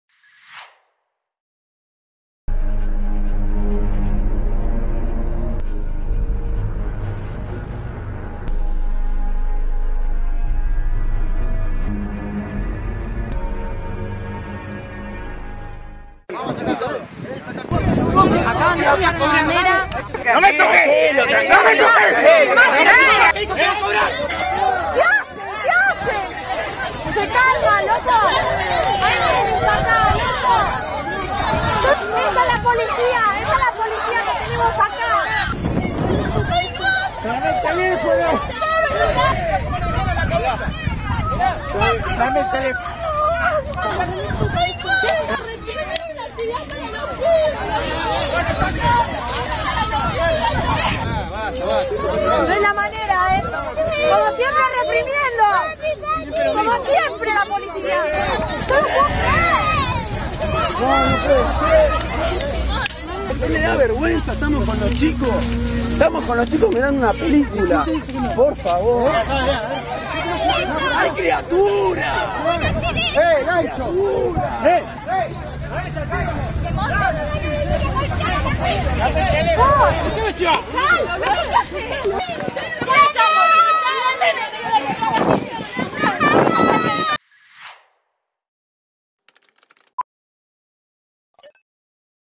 Represión a La Cámpora en San Isidro
Represión policial en San Isidro a militantes de La Cámpora mientras realizaban una actividad cultural en un espacio público.